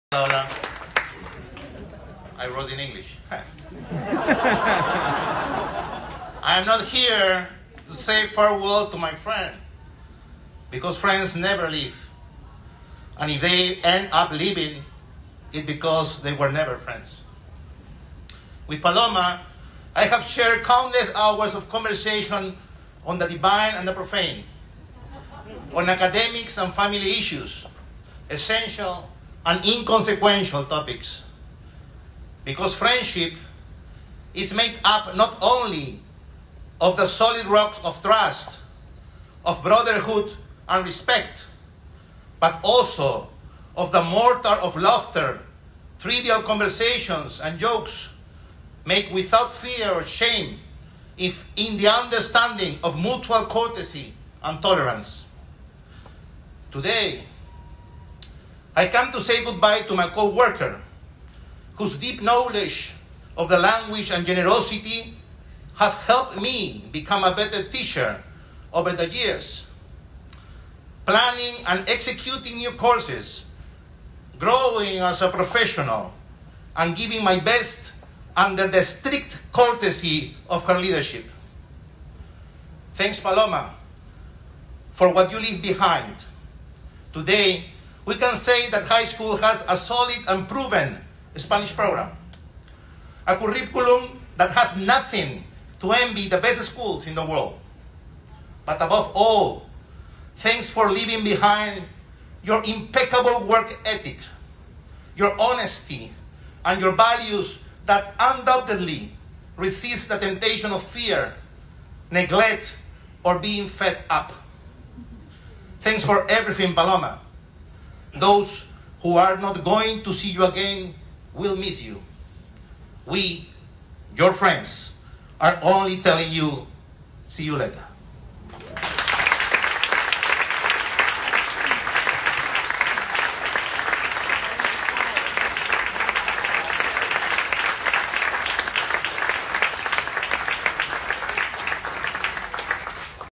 Farewell to Paloma Julián
I'm not here to say farewell to my friend, because friends never leave (and if they end up leaving, it's because they were never friends).

With Paloma, I have shared countless hours of conversation on the divine and the profane, on academics and family issues, essential and inconsequential topics, because friendship is made up not only of the solid rocks of trust, of brotherhood and respect but also of the mortar of laughter, trivial conversations and jokes made without fear or shame, in the understanding of mutual courtesy and tolerance.

Today I come to say goodbye to my co-worker, whose deep knowledge of the language and generosity, have helped me become a better teacher over the years, planning and executing new courses, growing as a professional and giving my best under the strict courtesy of her leadership.

Thanks, Paloma, for what you leave behind. Today we can say that HS has a solid and proven Spanish program, a curriculum that has nothing to envy the best schools in the world. But, above all, thanks for leaving behind your impeccable work ethic, your honesty and your values ​​that undoubtedly resist the temptation of fear, neglect or being fed up.

Thanks for everything, Paloma, those who are not going to see you again, will miss you; we, your friends, are only telling you "see you later".

Despedida a Paloma

No vengo a despedir a la amiga, porque los amigos nunca se van (y si acaban yéndose es que nunca estuvieron).

Con Paloma he compartido innumerables horas de conversación sobre temas divinos y profanos, académicos y familiares, imprescindibles e intrascendentes, porque la amistad está hecha no solo de las sólidas rocas de la confianza, la hermandad y el respeto sino también de la argamasa de las risas, las conversaciones triviales y las bromas lanzadas sin miedos ni verguenzas, en el entendimiento de la mutua cortesía y tolerancia.

Hoy vengo a decirle adiós a la compañera de trabajo, gracias a cuyo conocimiento profundo de la lengua y a su generosidad, me convertí en estos años, planeando y ejecutando nuevos cursos, en un mejor profesor.

Gracias, Paloma, por lo que nos dejas, porque bajo tu liderazgo hoy podemos decir que tenemos un programa de español sólido y probado, un curriculum que no tiene nada que envidiarle a los mejores colegios del mundo y, sobre todas las cosas, un ejemplo del Ser, una ética laboral intachable, una honradez nítida y unos valores que resisten impertérritos la tentación del miedo, la dejadez y el hartazgo.

Gracias por todo, Paloma, y que te extrañen los que no van a volver a verte, tus amigos solamente te decimos hasta luego.